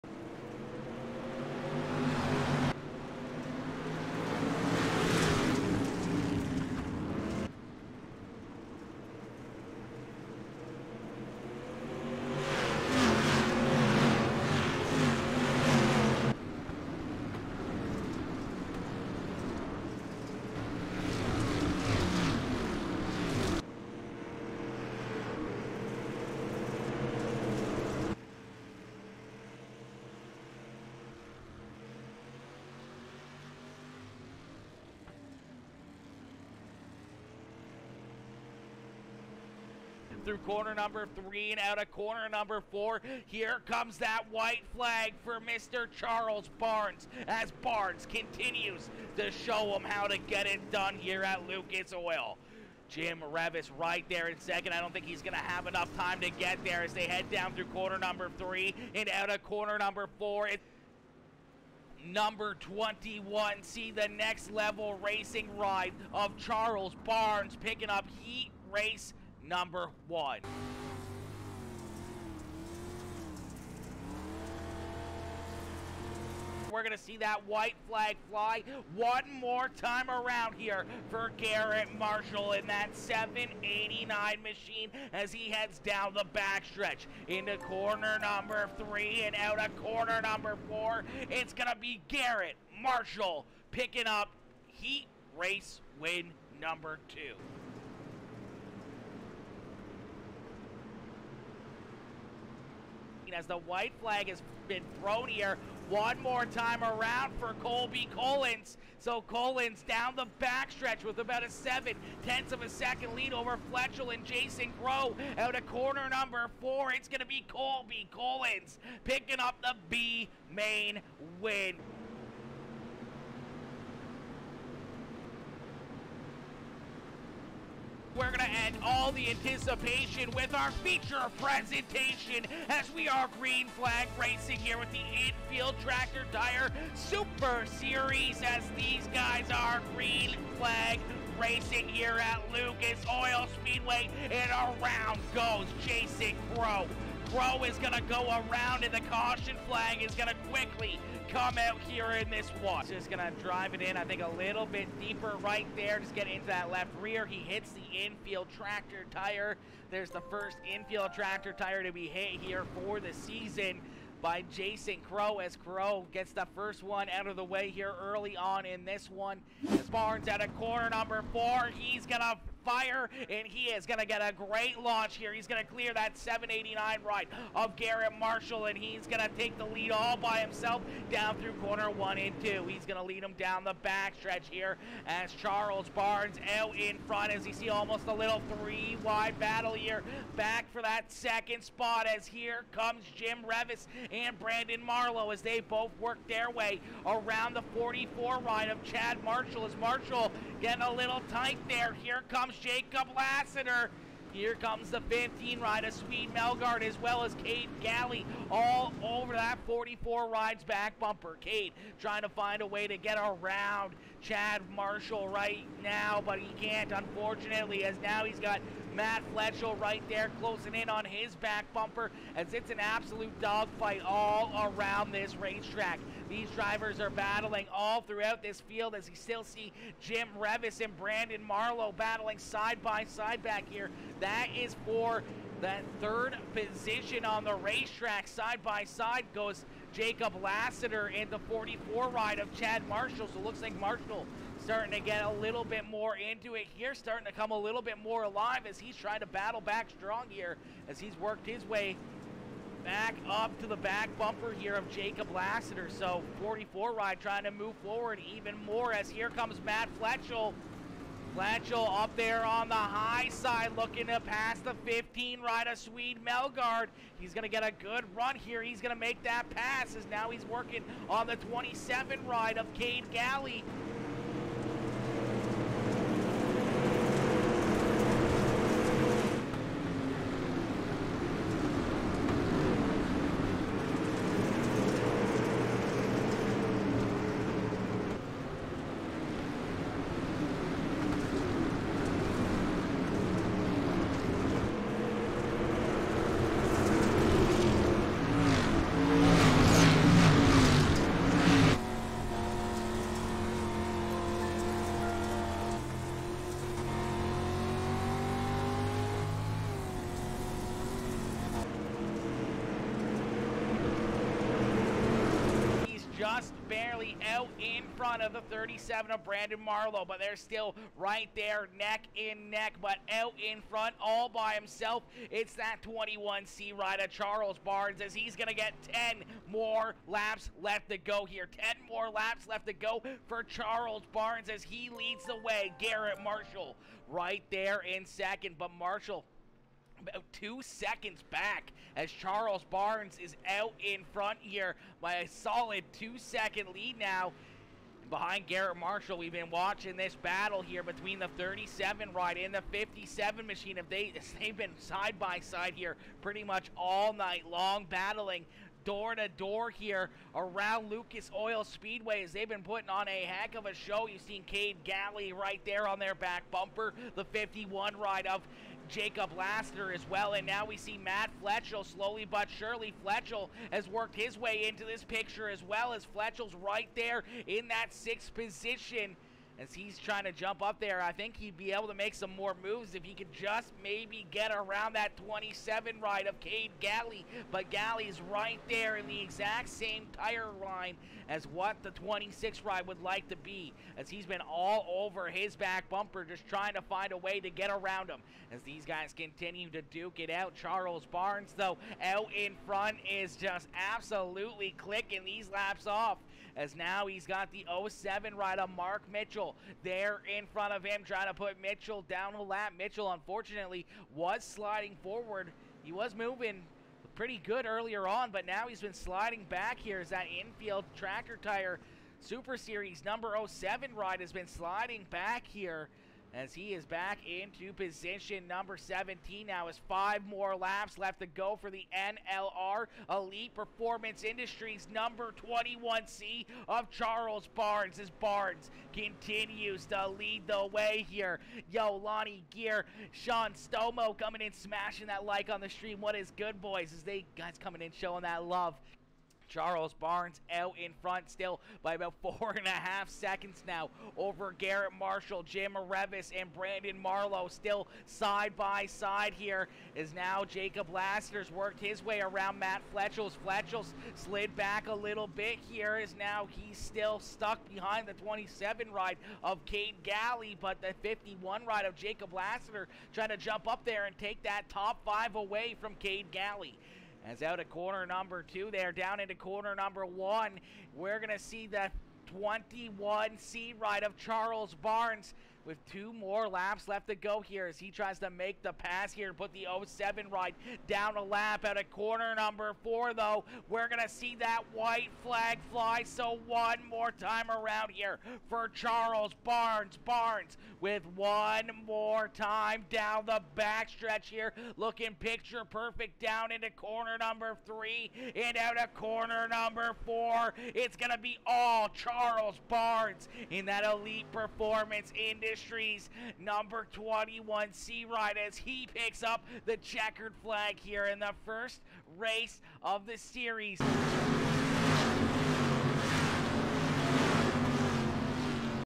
Infield Tractor Tire Super Series Night #1 Replay!
The Street Stocks headed into Lucas Oil Speedway for another jam packed night of racing action. This was the first race of the new season for the Infield Tractor Tire Super Series and these drivers put on a show!
Right from the first green flag down to the checkered we were all sitting at the edge of our seats.
Was it a good edit and made it worth watching? Was it to long or to short?
Let us know what your thoughts are!
Peace & Love!